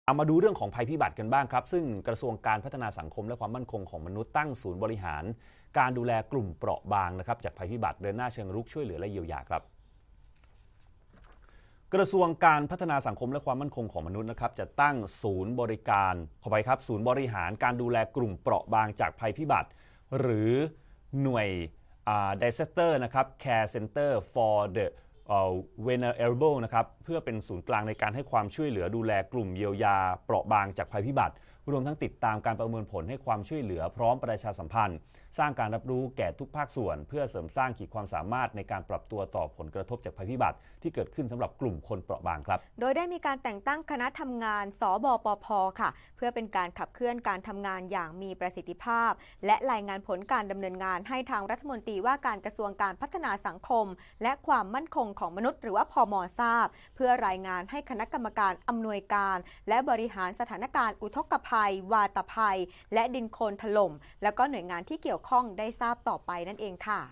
พม.ตั้งศูนย์บริหารการดูแลกลุ่มเปราะบางจากภัยพิบัติ
กระทรวงการพัฒนาสังคมและความมั่นคงของมนุษย์ (พม.)  ตั้งศูนย์บริหารการดูแลกลุ่มเปราะบางจากภัยพิบัติ เดินหน้าเชิงรุกช่วยเหลือ เยียวยา